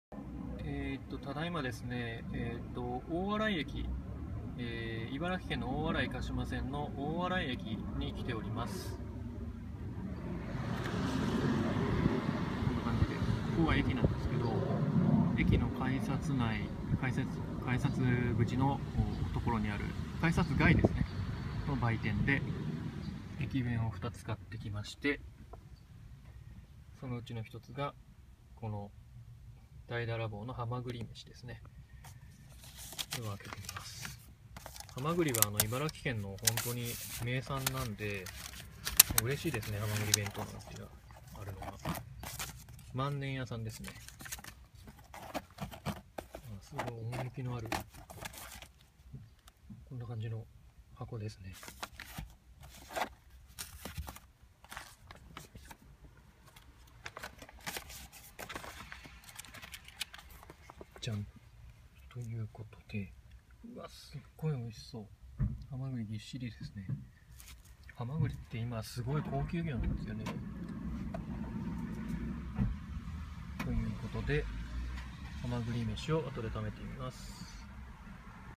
大洗駅のはまぐりめしを開けてみた
鹿島臨海鉄道・大洗鹿島線の大洗駅にて、美味なる駅弁「はまぐりめし」を購入して開けてみた時の記録です。幸せだ～。